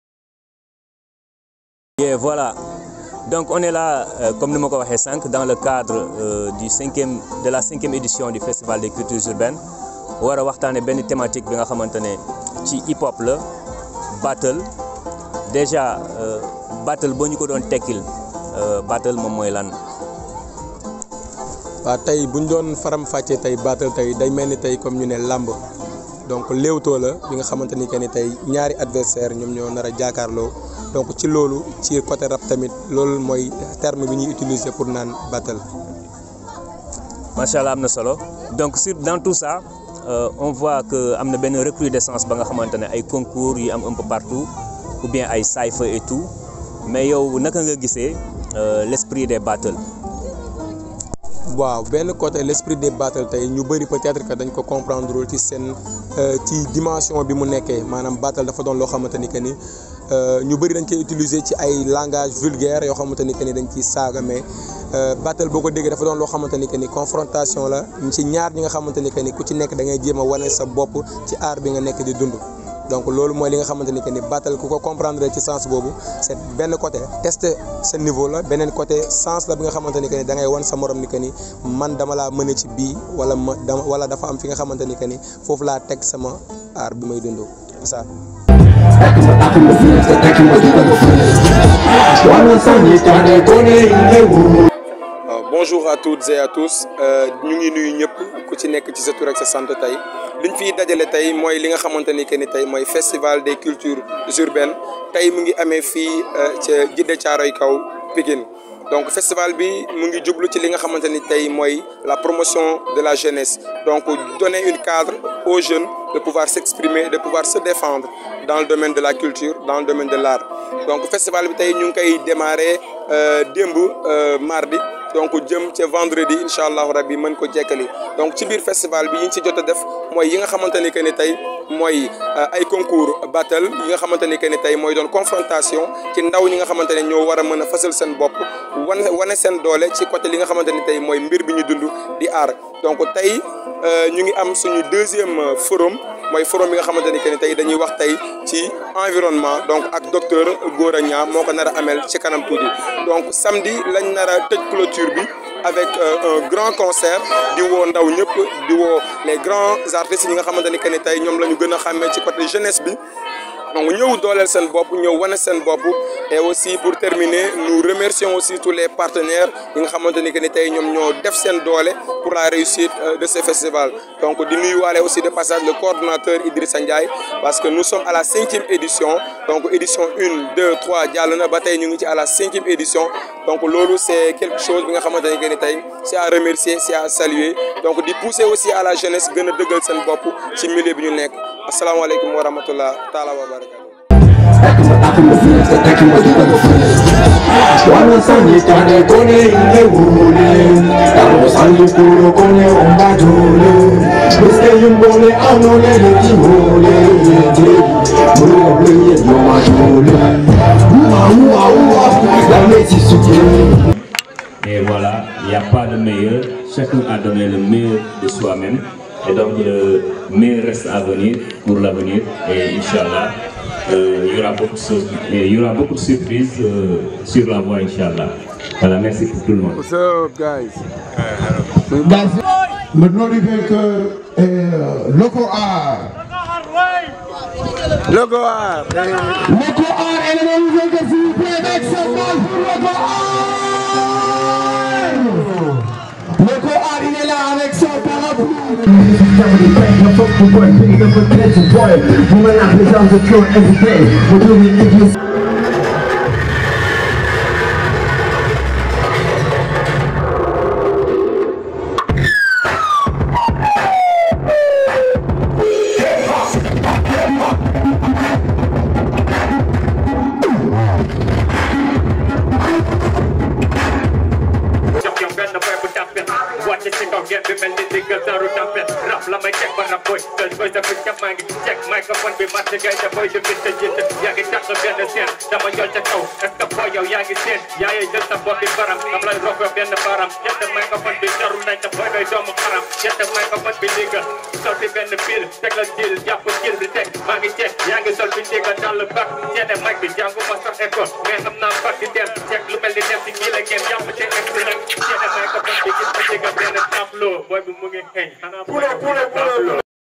5e EDITION FESTIVAL DES CULTURES URBAINES
Cette video est un extrait de la 5e édition du festival des cultures Urbaines, organisé par l'association Africa Cocktail du  13 au 17 Décembre à Pikine dans la Commune de djeddah Thiaroye kaw